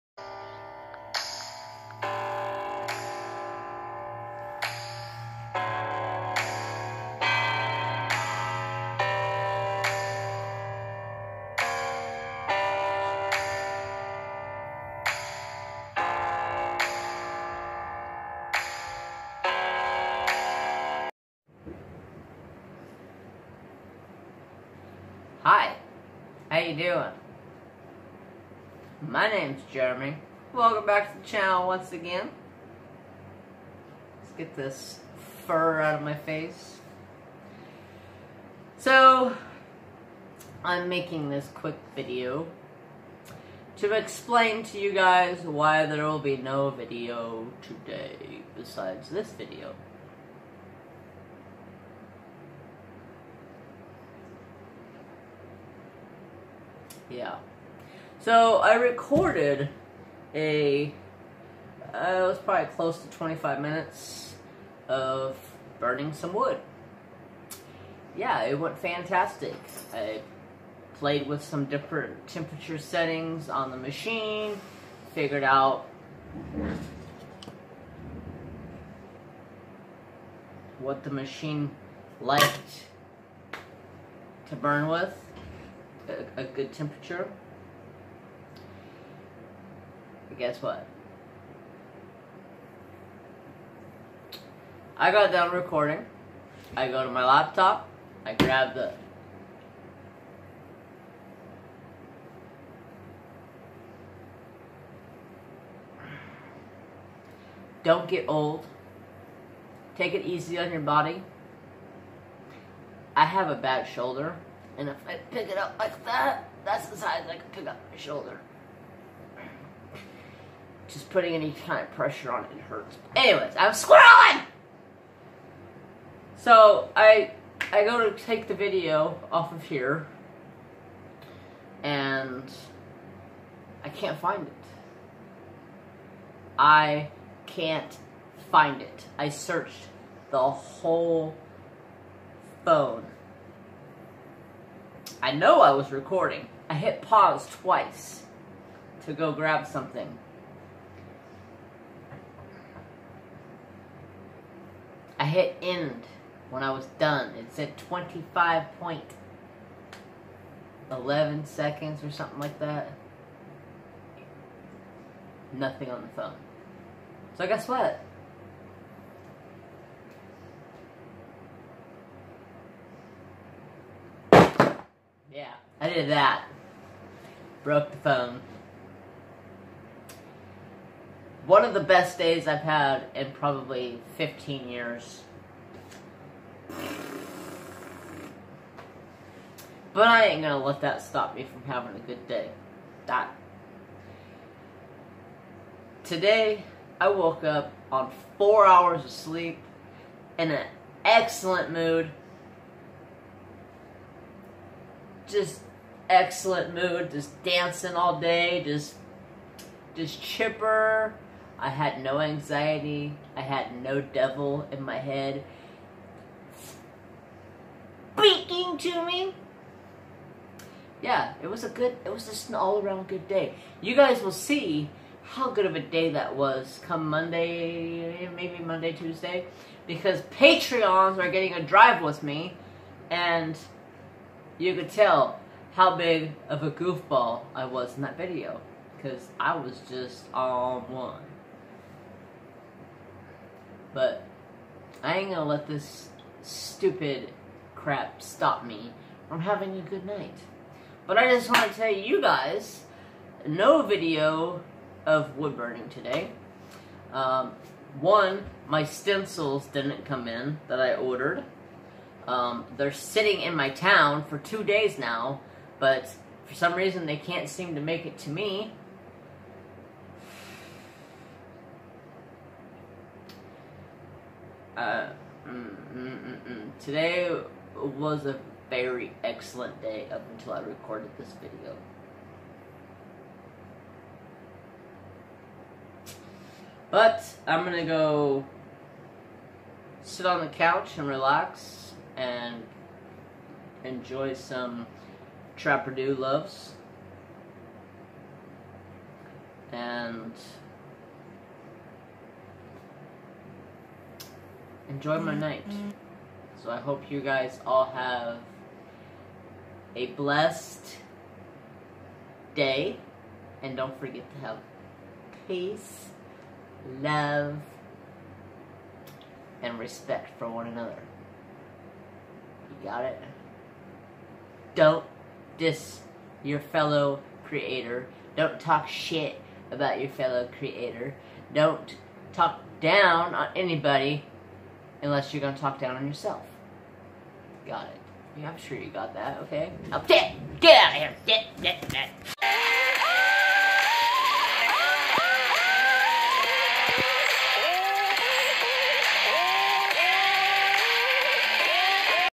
This is easier than a community tab post sorry for not having a normal crafty video today
Recorded a wood-burning video last night and my phone decided to eat the video so I got pissed off I threw my phone on the desk and broke it so you guys get the short video today and I hopefully will be fixed by today or tomorrow so new phone time have a great day